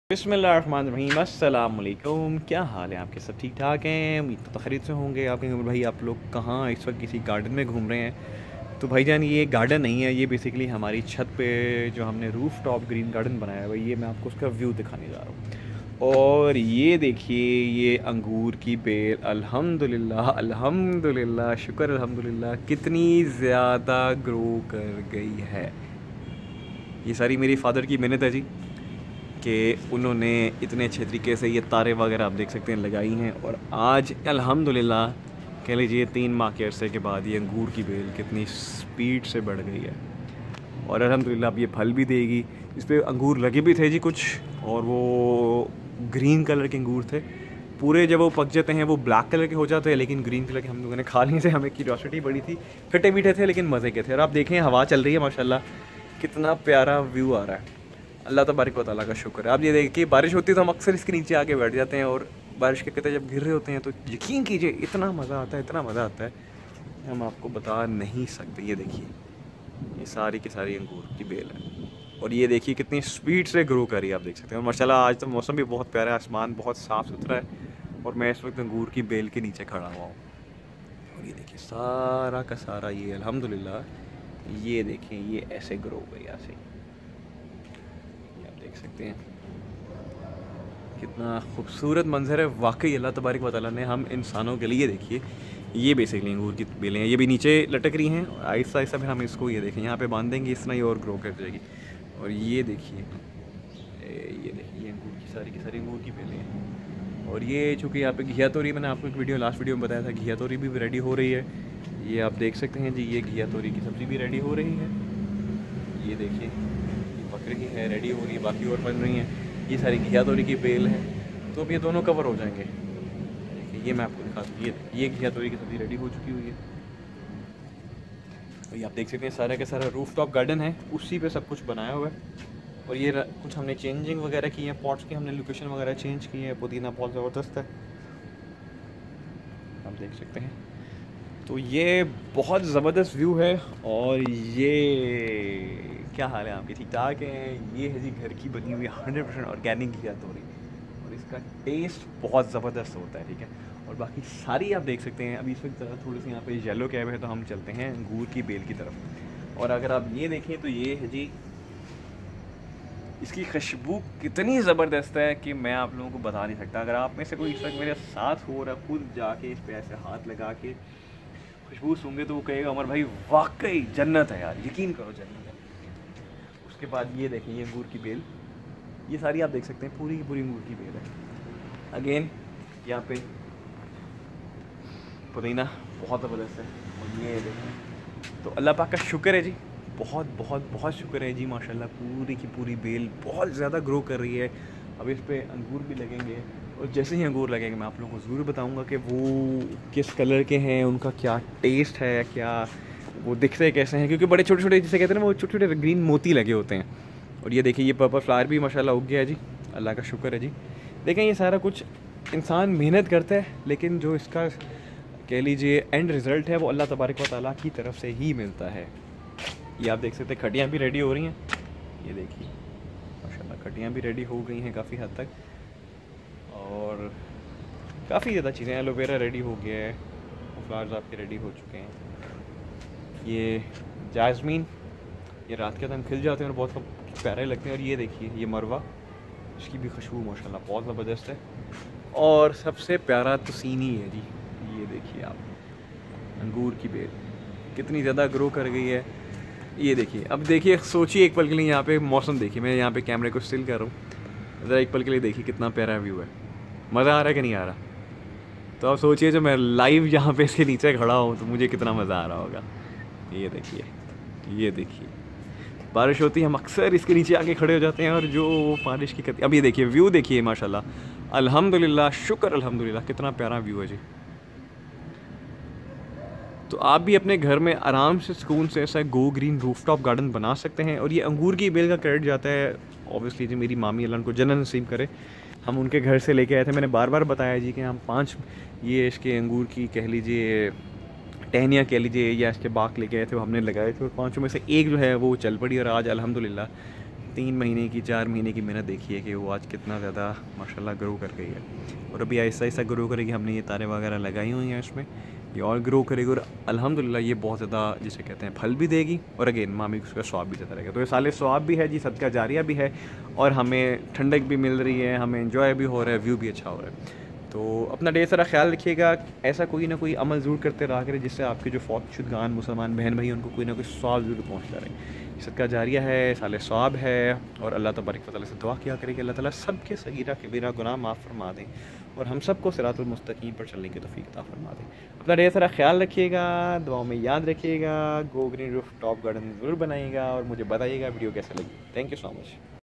How to grow Grapes at home? Angoor ki bail ghar pr kaise ugaye? Ghar ki Chhat pr Angoor kaise ugaye?
How to grow Grapes at home? Angoor ki bail ghar par kaise ugaye? Ghar ki Chhat par Angoor kaise ugaye?